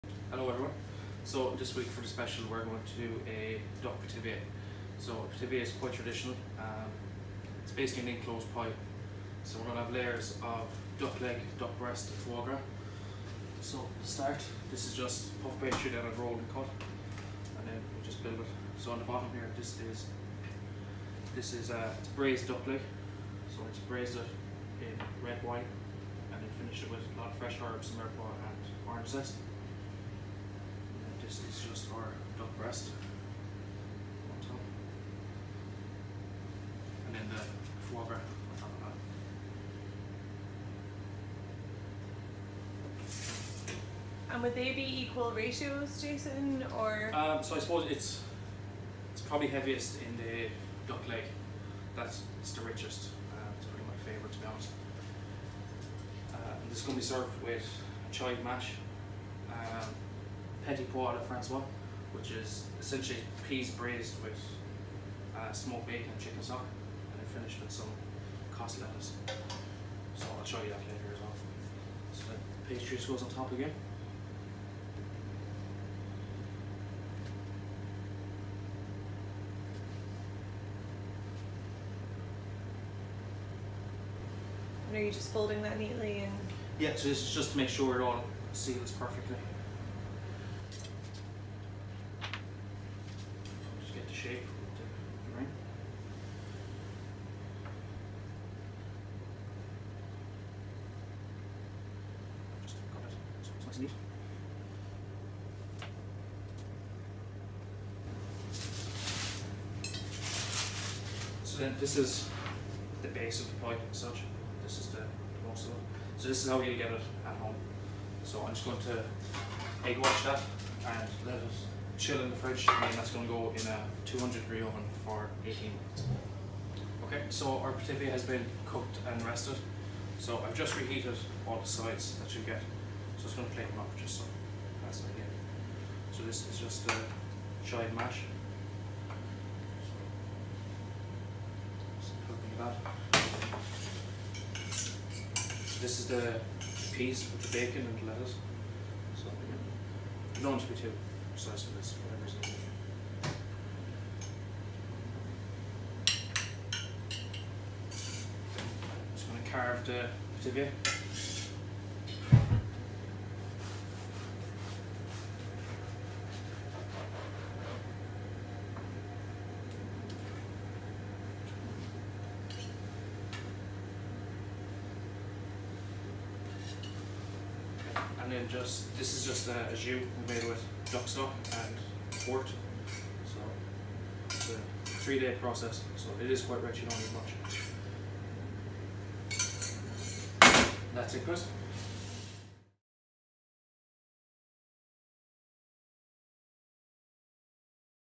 Duck Pithivier Weekend Special
Yes it’s that time of the week again — OUR WEEKEND SPECIAL! This week we’re all about the Pithivier — a Duck & Foie Gras Parcel of deliciousness! Available this Friday & Saturday for €19.95 per person which includes chive mash, Petit Pois de Francaise & a Port jus! Add a bottle of the house white or red to make it €27.50!

Order from 4pm on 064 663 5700, limited availability